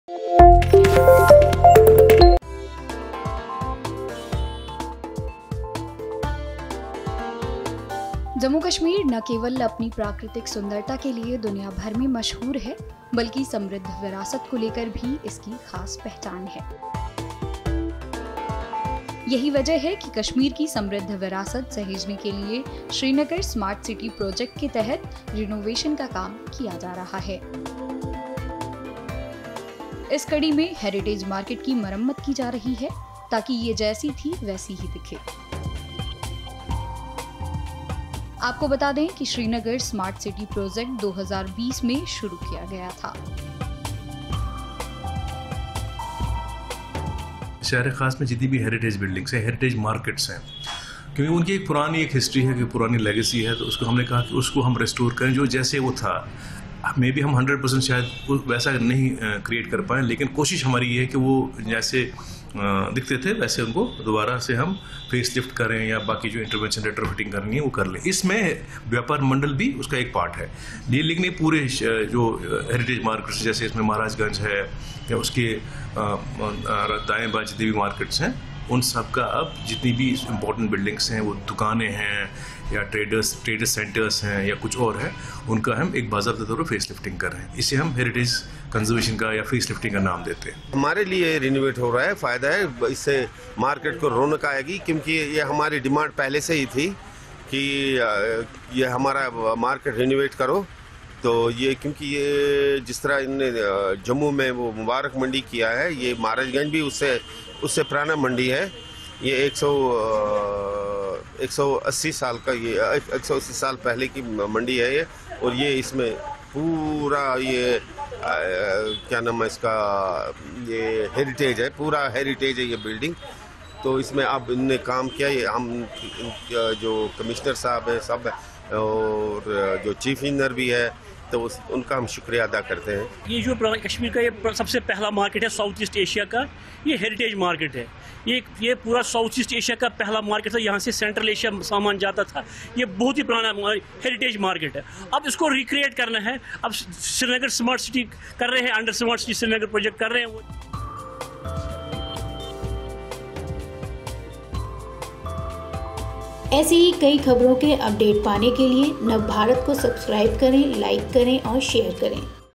Shrinagar Smart City Project के तहत कश्मीर की विरासत को सहेजने का काम जोरों पर
जम्मू-कश्मीर न केवल अपनी प्राकृतिक सुंदरता के लिए दुनियाभर में मशहूर है बल्कि समृद्ध विरासत को लेकर भी इसकी खास पहचान है। यही वजह है कि कश्मीर की समृद्ध विरासत सहेजने के लिए श्रीनगर स्मार्ट सिटी प्रोजेक्ट के तहत रिनोवेशन का काम किया जा रहा है।